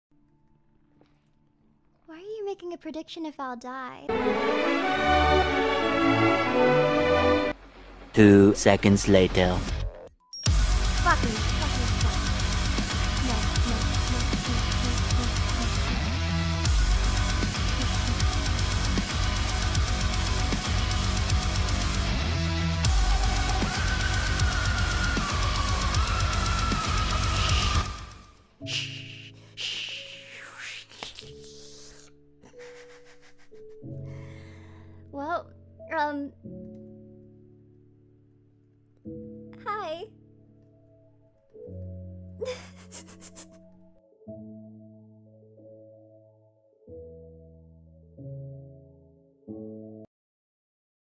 Dragon tries to conquer the nether
Sooo our dragon now tries the nether... and it went bad